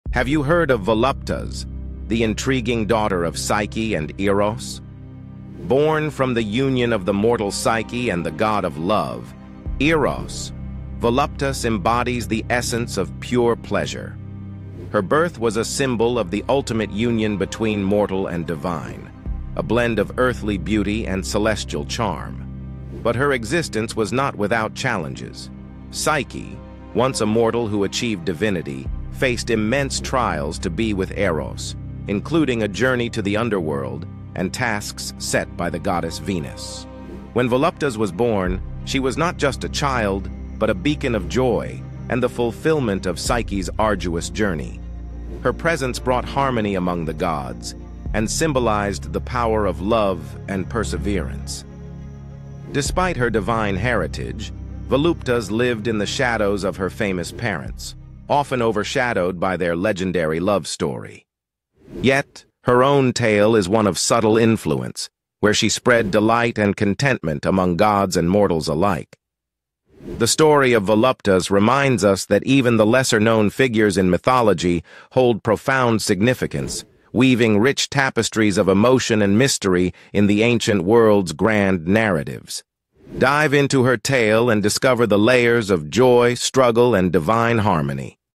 The Mystery of Psyche and Eros' Daughter
Unravel the enigmatic tale of Voluptas, the daughter of Psyche and Eros. Dive into the depths of ancient mythology and discover a story filled with drama, mystery, and emotion.